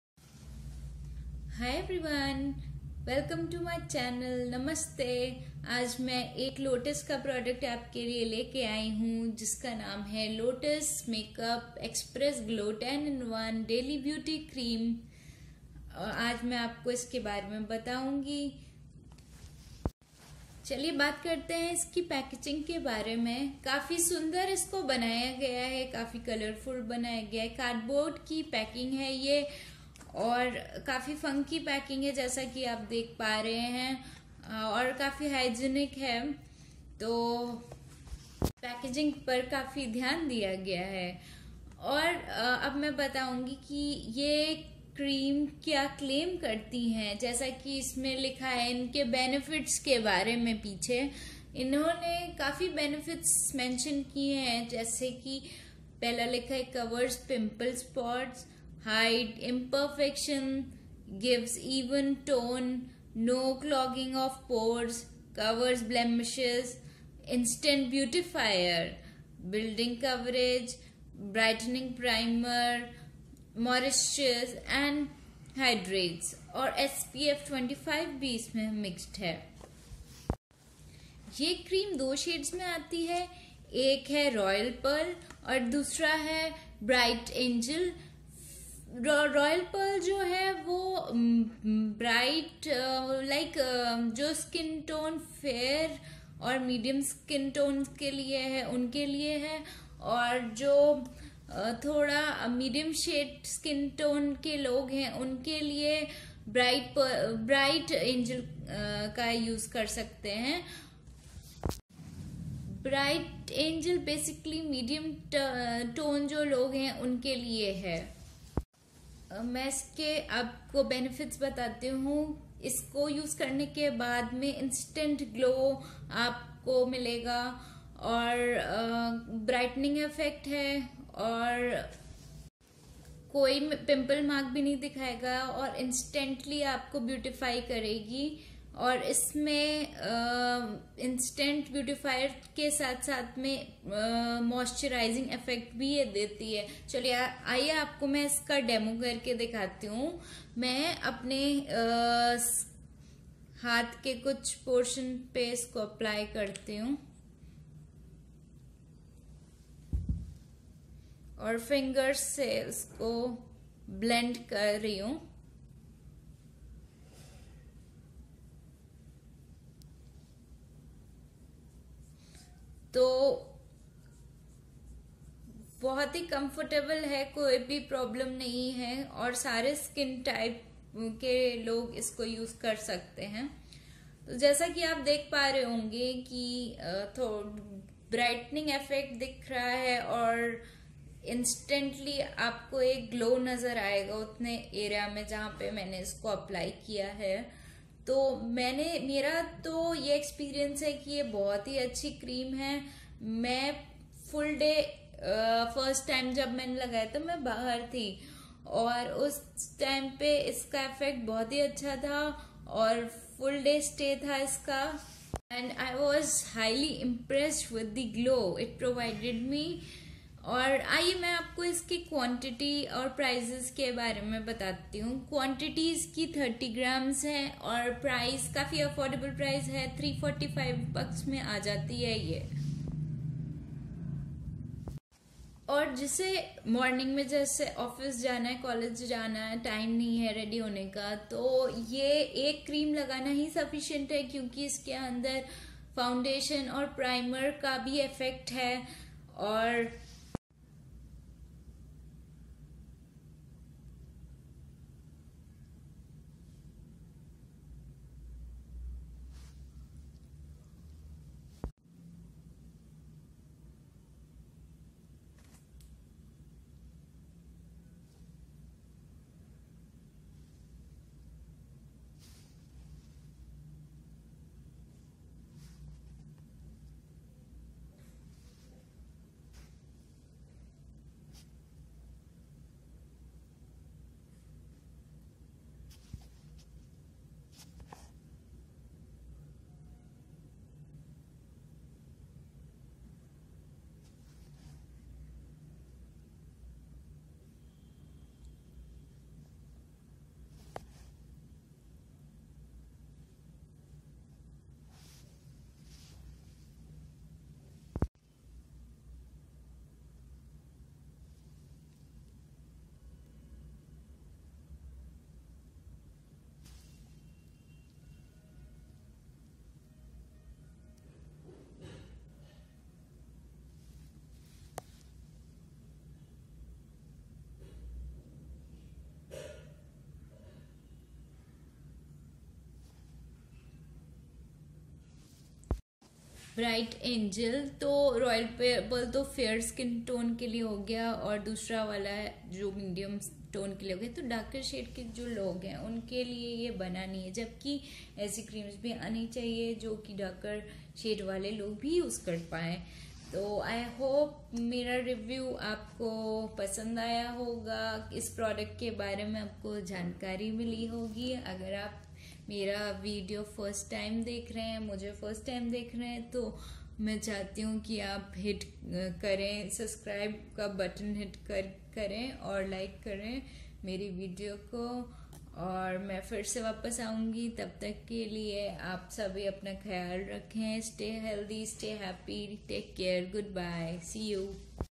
Lotus Make-Up Xpress Glow 10-in-1 Daily Beauty Creme SPF - 25 Use, Price, Review in Hindi
Benefits of Lotus Make-up Xpress Glow Daily Beauty Crème

Made from natural active ingredients this beauty crème is a must have for those who want to look good naturally. This unique formulation consists of:

Licorice Extracts
Licorice extracts have a natural active ingredient called glabridin which helps reduce skin discoloration and appearance of dark spots. Licochalcone balances oil levels in your skin and protects it from environmental stress.

Soothing Aloe
AloeVera delivers gentle moisturizing care to your skin and naturally reduces the occurrence of dark spots, making your skin look even toned and radiant!